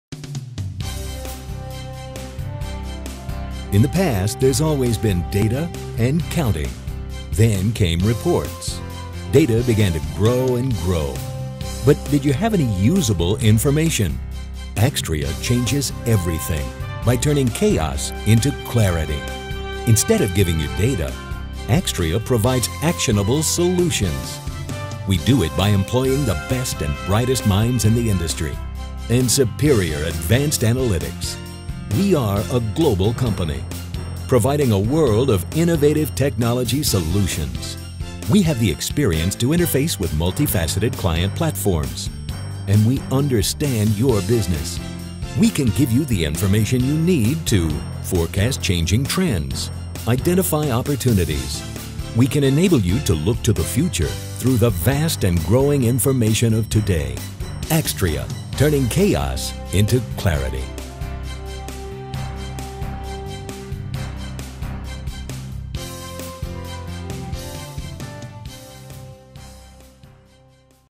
Axtria Introduction
Axtria is a Business Analytics company. We combine business knowledge, analytics and technology to provide product enabled analytics services to customers.

We are experts in Big Data integration, management and advanced analytics with strong business domain knowledge.